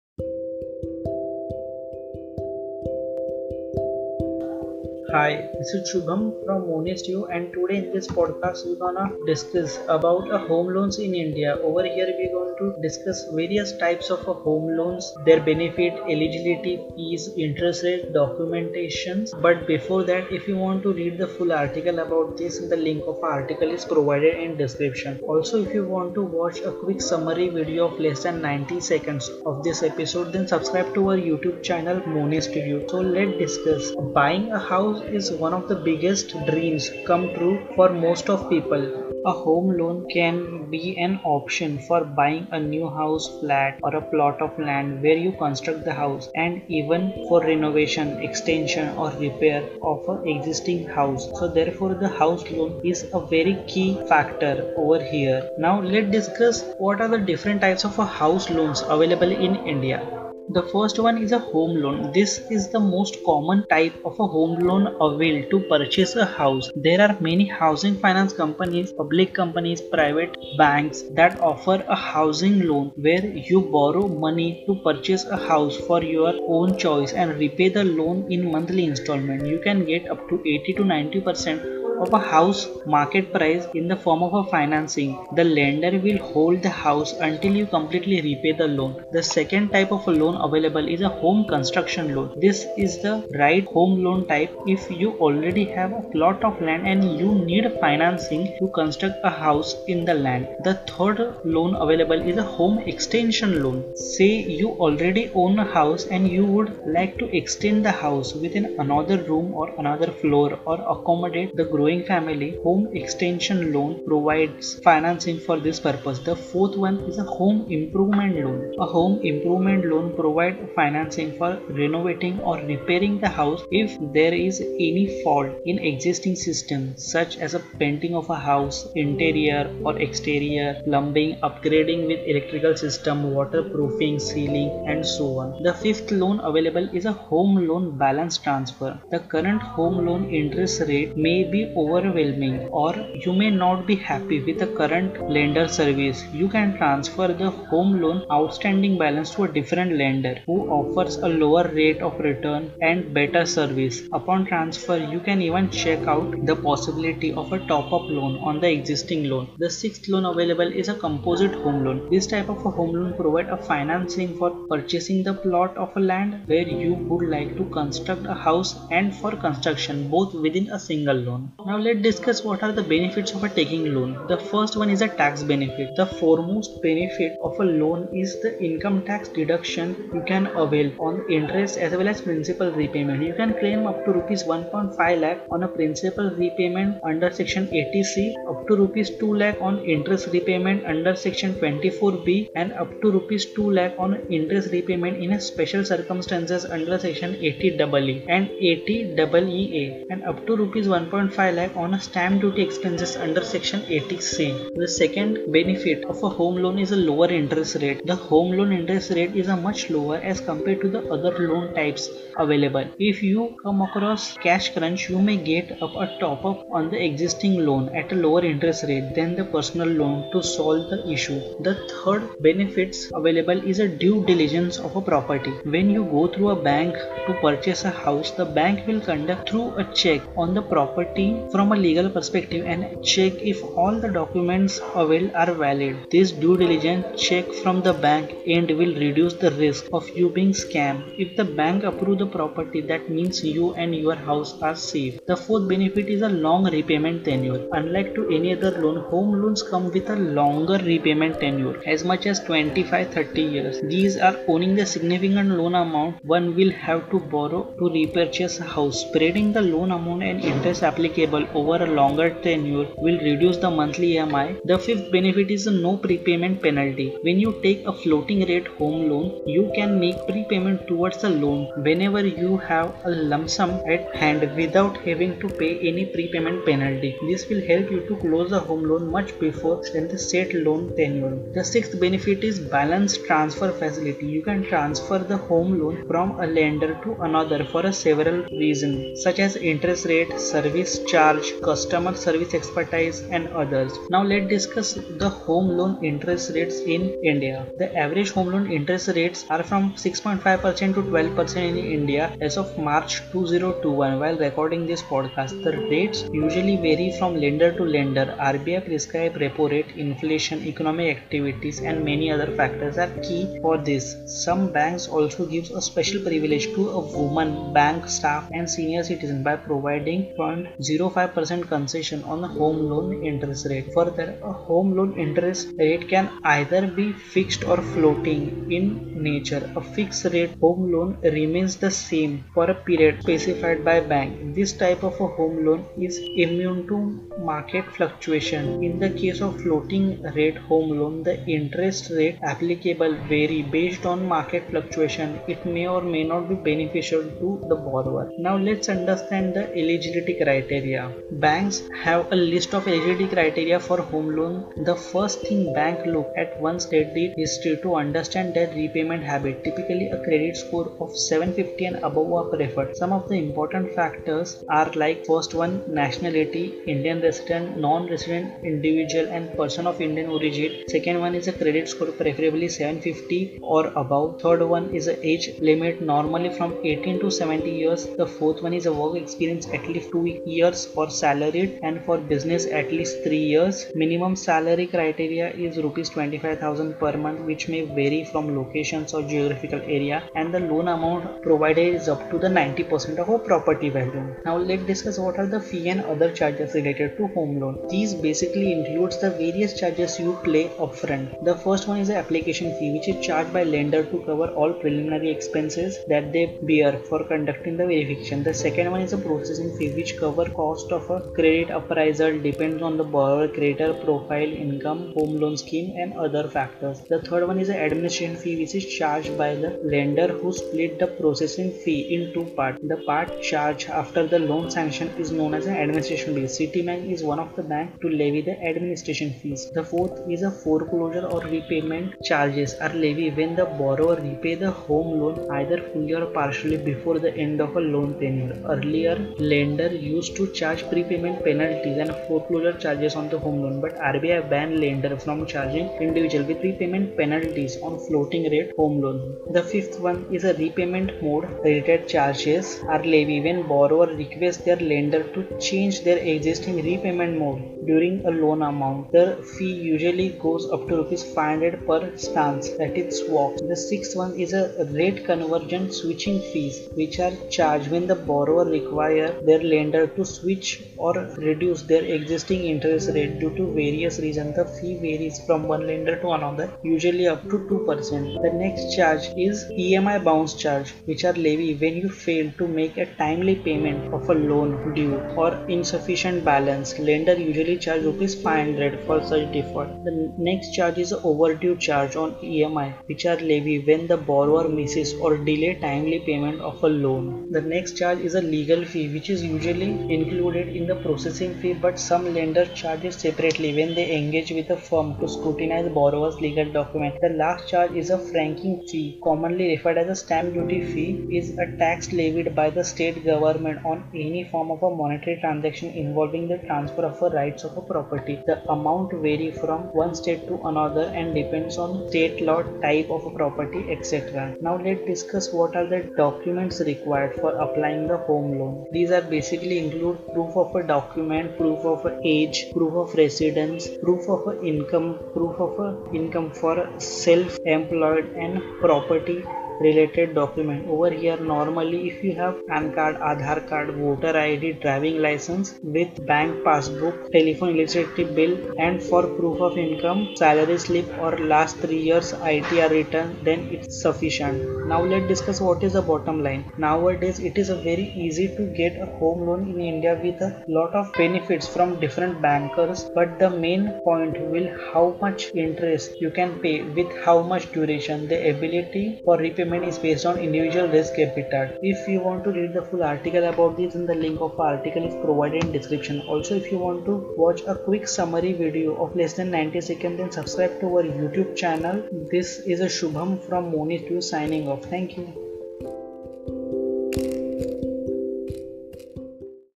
EP 46 Home loan in India - A Complete Guide
In this podcast we will discuss about Home loan in India - it's Meaning, Types, Benefits, Eligibility, Fees, Interest rates, Documentation & Bottomline

Timestamp
00:00 Introduction
00:33 Why Home Loans ?
00:56 Types of Home Loans
02:52 Benefits of Taking a Home Loan
04:54 Latest Interest Rates
05:48 Eligibility
06:35 Fees & Other Charges
08:45 Documentation
09:15 Bottomline

2. Amazon (kindle)

1. Amazon (Asus Laptop's)
2. Amazon (Tablor's Mic)
3. Amazon (Tygot's LED lights)